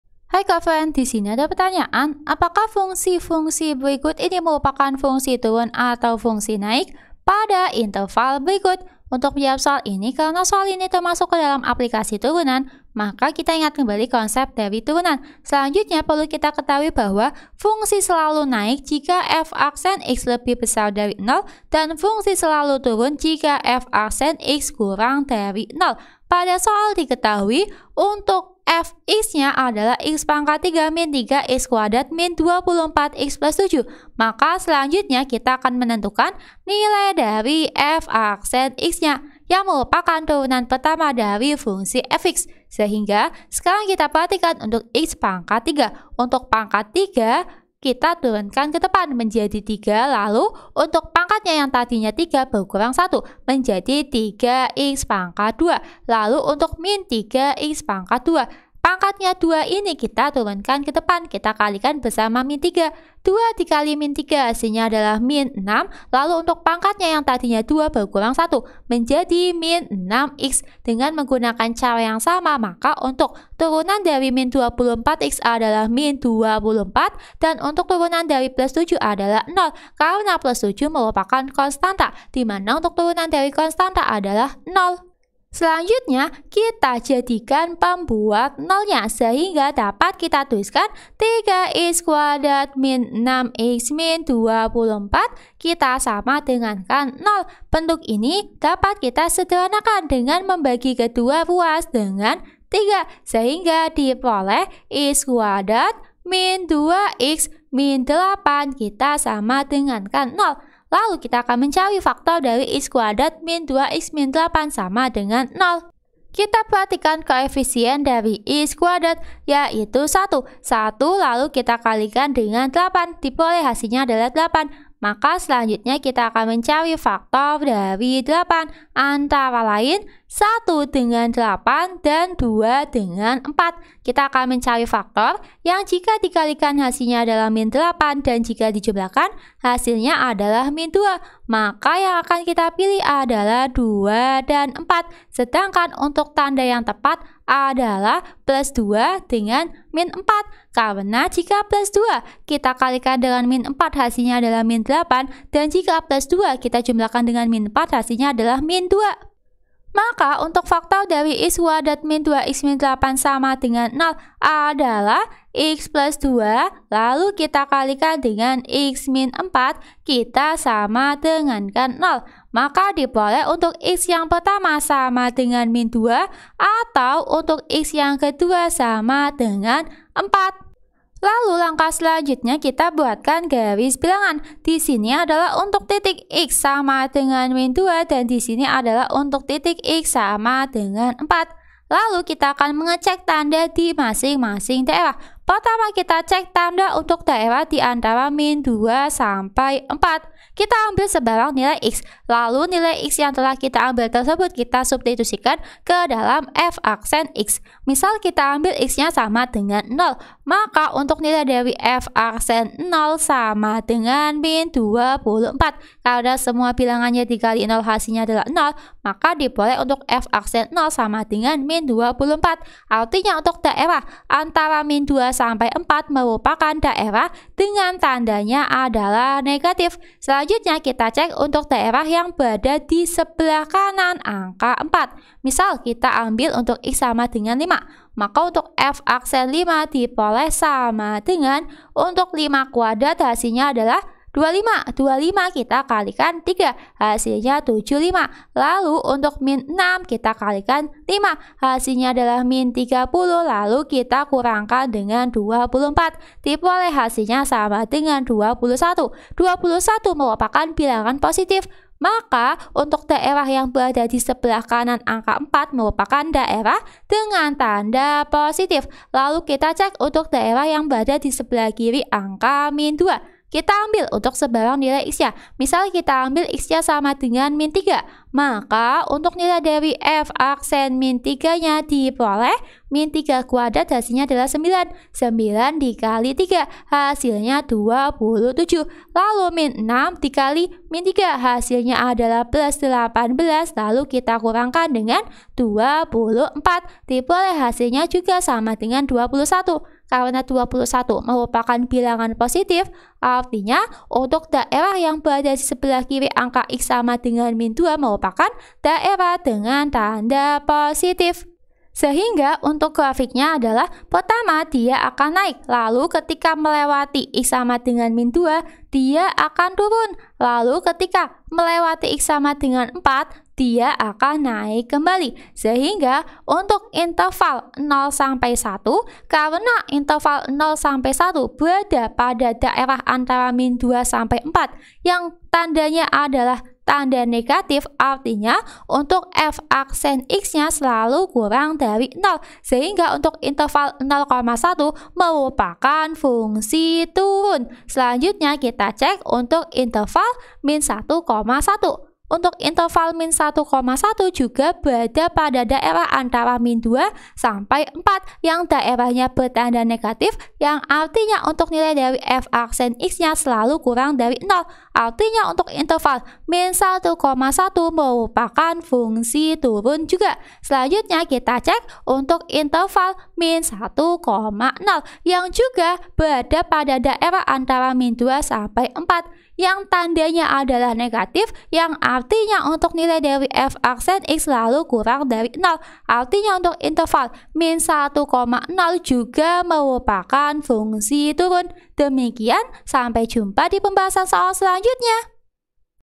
Tunjukkan apakah fungsi-fungsi berikut merupakan fungsi turun atau fungsi naik pada interval ...
Tunjukkan  apakah fungsi-fungsi berikut merupakan  fungsi turun atau   fungsi naik pada interval [0, 1], [-1, 1], [-1, 0j. f(x) = x 3x3 24x + 7

Topik atau Materi: Fungsi Naik - Turunan - KALKULUS - Matematika Kelas 11 SMA - K-13 revisi

Sumber Soal: Pendalaman Matematika Wajib Kelas 11 Smt. 2, K-13 revisi, KND 2017 , Yudhistira

00:00 : Bedah Soal
01:00 : Konsep, rumus dan pengertian Fungsi Naik
01:18 : Langkah penyelesaian soal
05:35 : Jawaban akhir
06:05 : Penutup